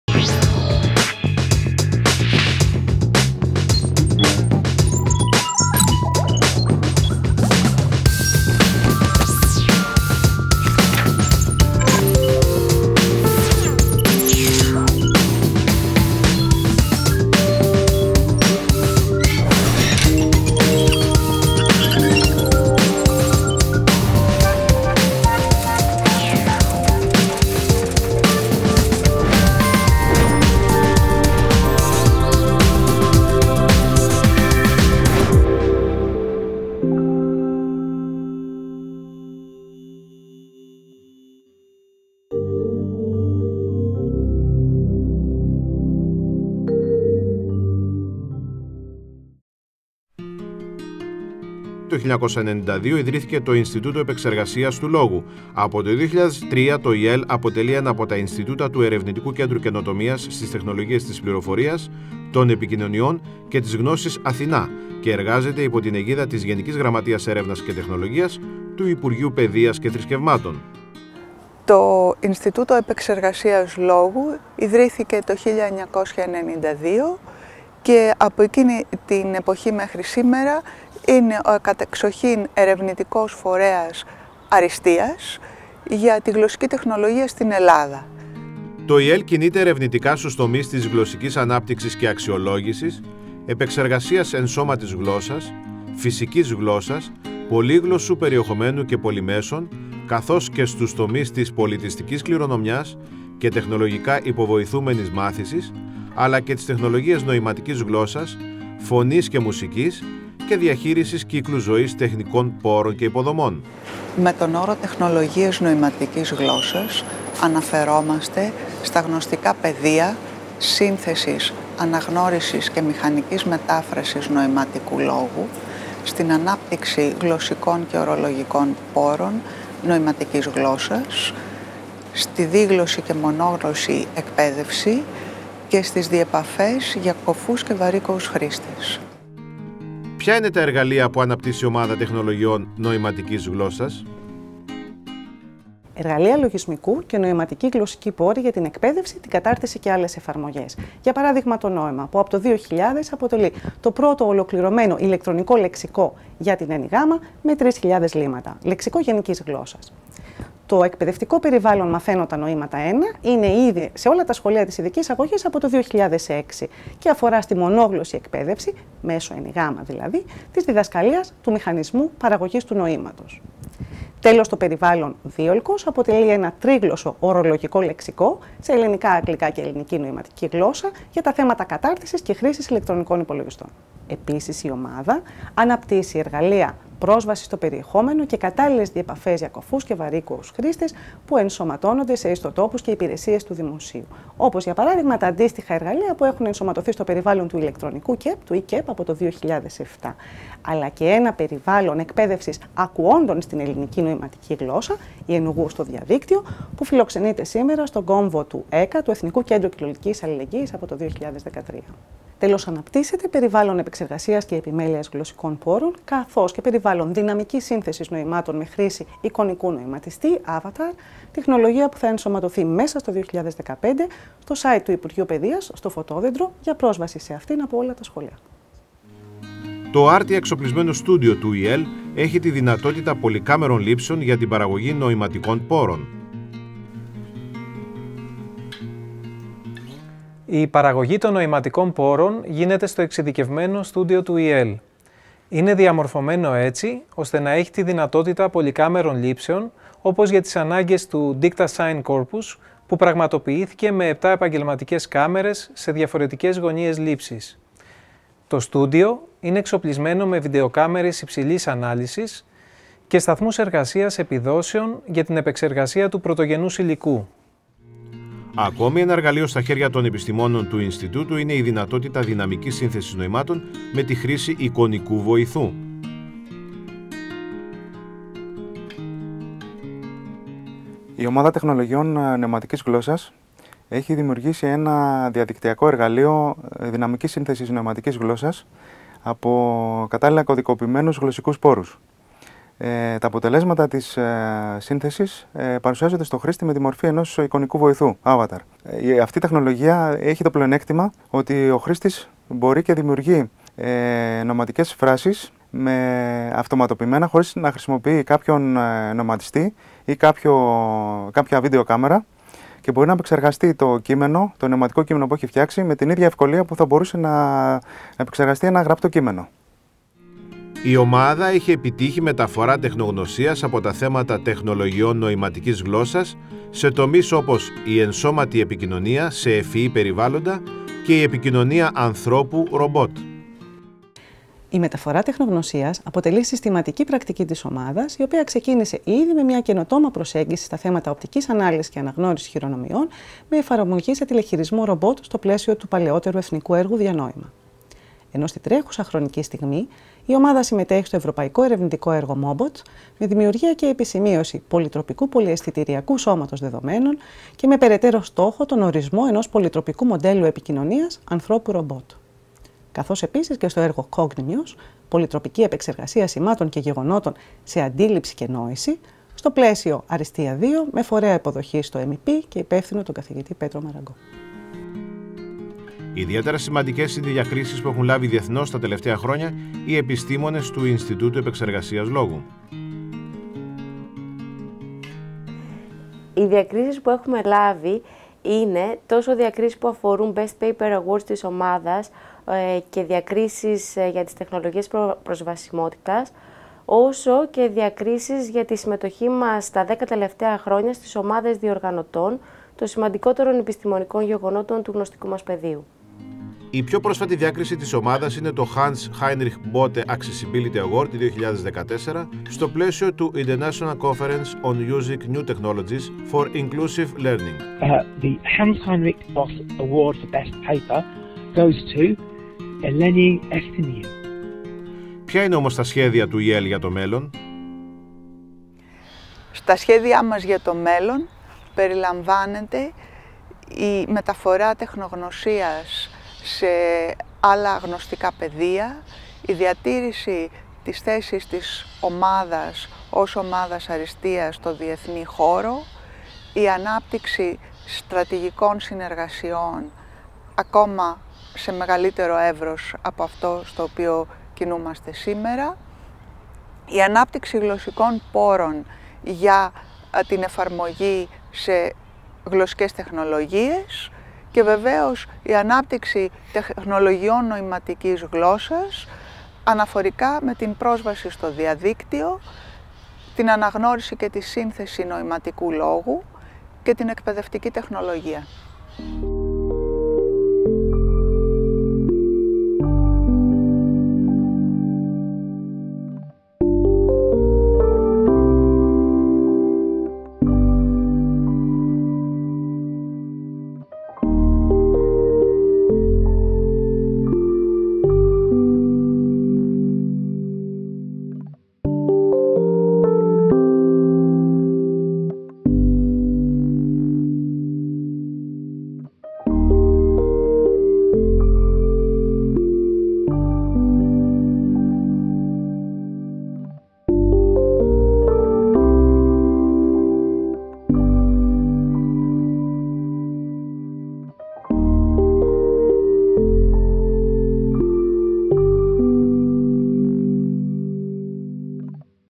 ΟΜΑΔΑ ΕΠΕΞΕΡΓΑΣΙΑΣ ΝΟΗΜΑΤΙΚΗΣ ΓΛΩΣΣΑΣ - ΙΕΛ / Ε.Κ. "ΑΘΗΝΑ"
Οι τεχνολογίες ενσώματης / πολυτροπικής επικοινωνίας εντάσσονται στις Γλωσσικές Τεχνολογίες. Βρίσκουν άμεση εφαρμογή σε εξελιγμένα συστήματα διάδρασης ανθρώπου-μηχανής, συμπεριλαμβανομένης και της επικοινωνίας ανθρώπου - ρομπότ, και ενσωματώνονται σε ευφυή περιβάλλοντα.

To Ινστιτούτο Επεξεργασίας του Λόγου (ΙΕΛ) αποτελεί από το 1992, τον κατ’ εξοχήν ερευνητικό φορέα στο χώρο της γλωσσικής τεχνολογίας, στην Ελλάδα. Από το 2003, είναι μέλος του Ερευνητικού Κέντρου Καινοτομίας στις Τεχνολογίες της Πληροφορίας, των Επικοινωνιών και της Γνώσης, «ΑΘΗΝΑ», υπό την αιγίδα της Γενικής Γραμματείας Έρευνας και Τεχνολογίας (ΓΓΕΤ) του Υπουργείου Παιδείας.